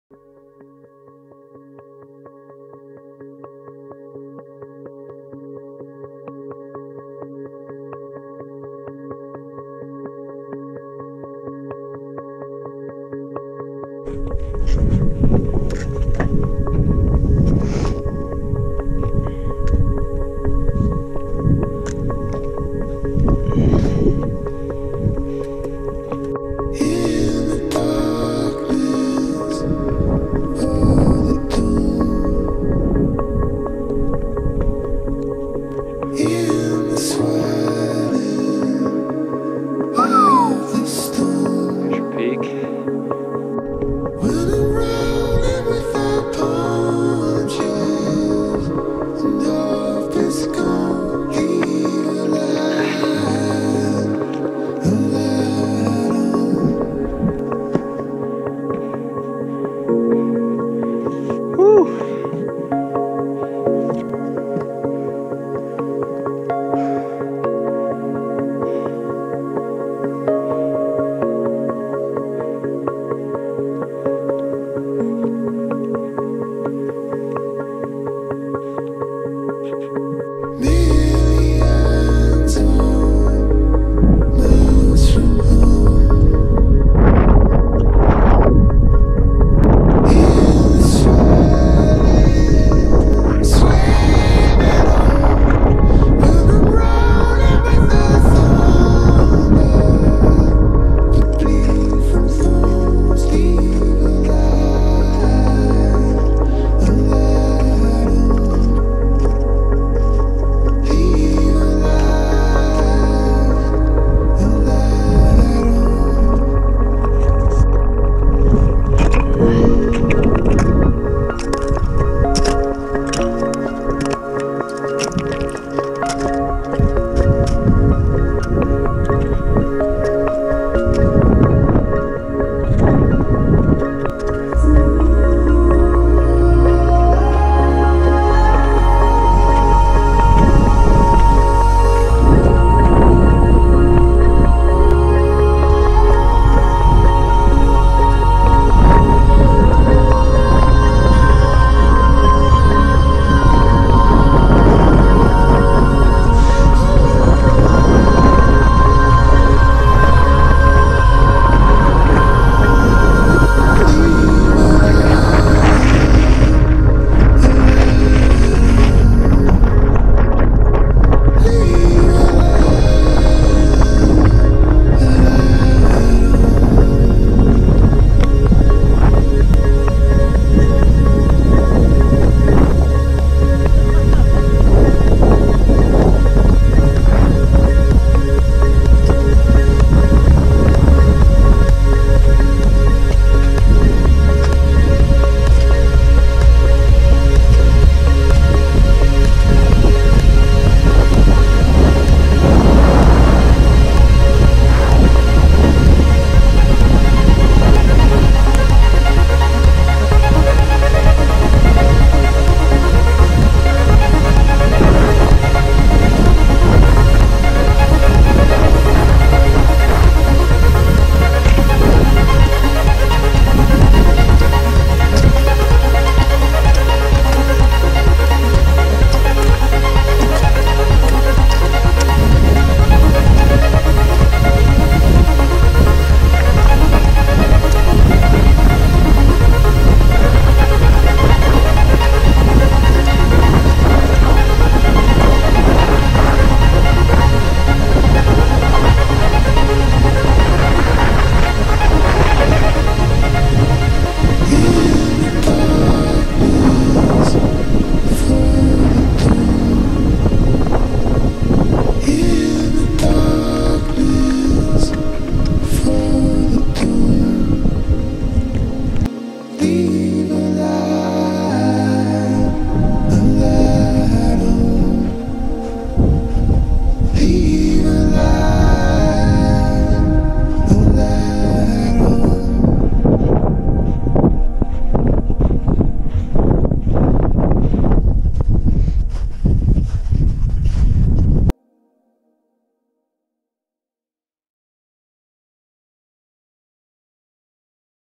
Climb of Mt. Hinman, July 2019
A summit of secluded, 7,492 ft Mt. Hinman in the Alpine Lakes Wilderness.